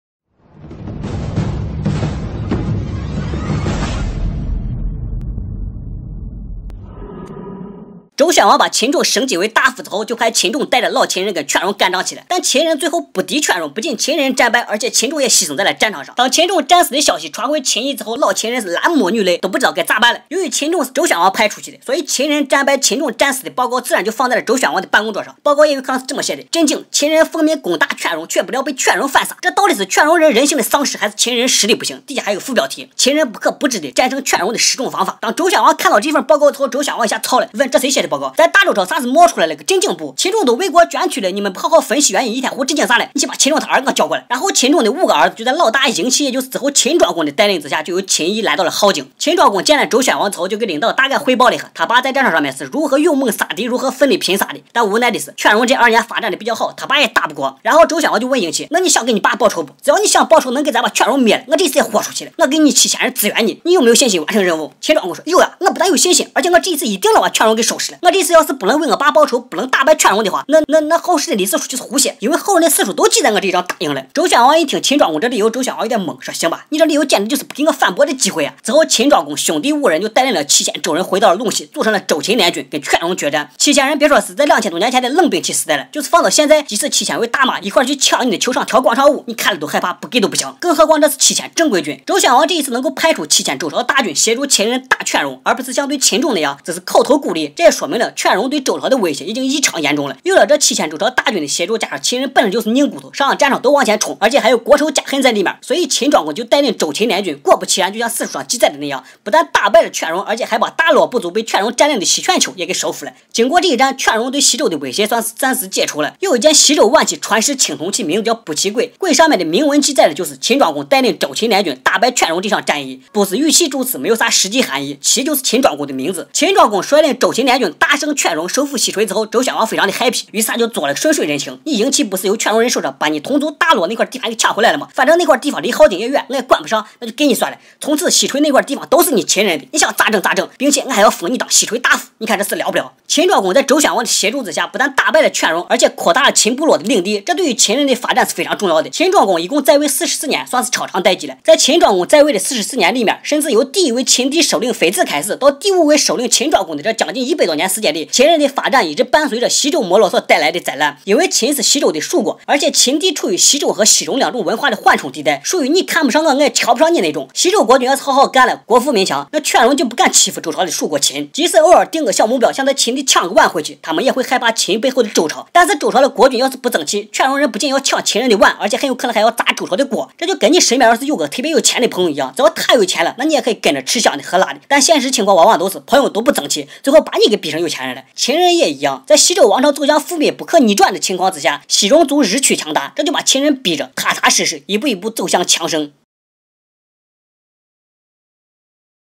第四期、秦庄公 Duke Zhuang of Qin
秦五粮陕西话三分钟讲完大秦帝国